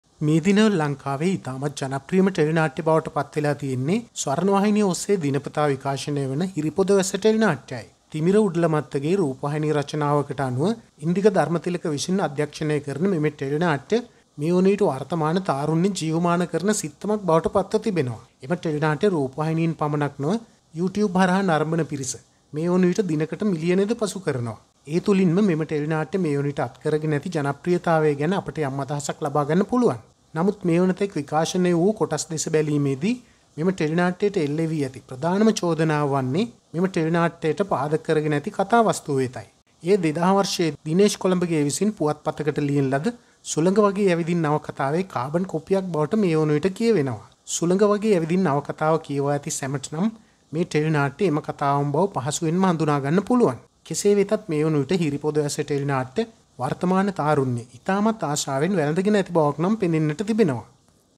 හිරිපොද වැස්ස ටෙලිනාට්‍ය සුලග වගේ ඇවිදින් නවකතාවේ කොපියක් /hiripoda wessa 35 /hiripoda wessa today
හිරිපොද වැස්ස ටෙලිනාට්‍ය සුලග වගේ ඇවිදින් නවකතාවේ කොපියක්
hiripoda wessa 35
hiripoda wessa today

මේ දිනවල ලංකාවේ ජනප්‍රිය ටෙලිනාට්‍ය වන හිරිපොද වැස්ස ටෙලිනාට්‍යට පාදක වී ඇත්තේ 2000 වර්ෂයේ පුවත්පත පලවූ සුලග වගේ ඇවිදින් නවකතාව බව කියවෙනවා ඒ පිළිබඳව වැඩිදුරටත් තොරතුරු ඔබට දැන ගන්න පුලුවන්

We Use Voice for Background Commentary of this video in Our language sinhala.